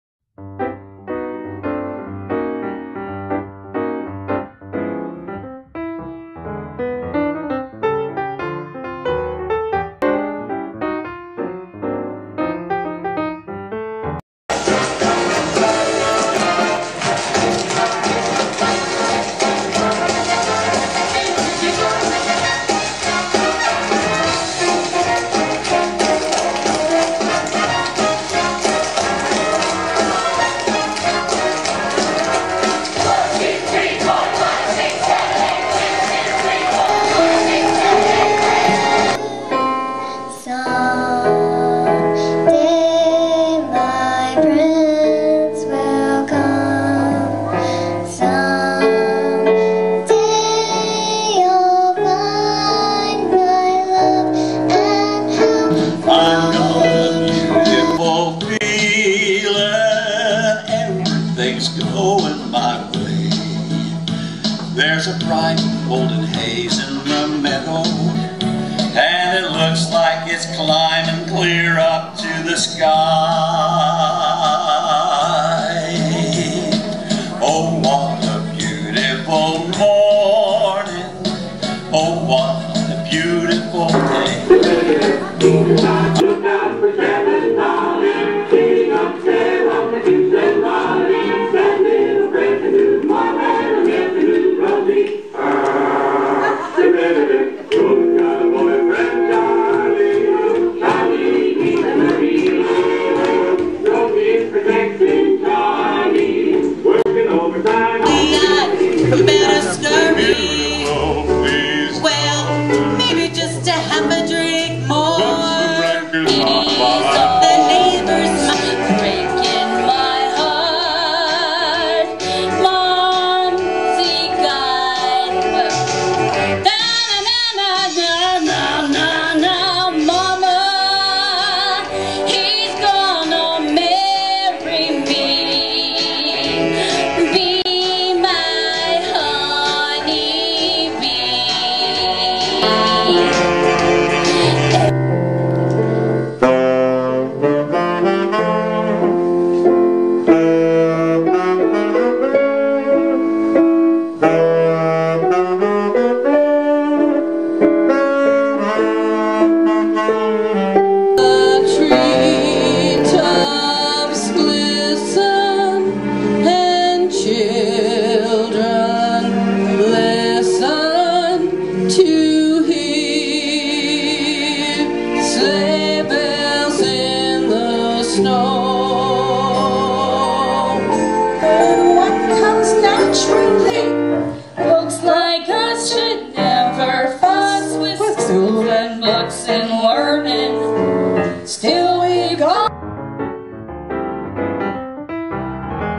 No Biz Like Show Biz
Mesquite Arts Council present the first show of the 2009 year 'No Biz Like Show Biz' as a salute to the 1940's.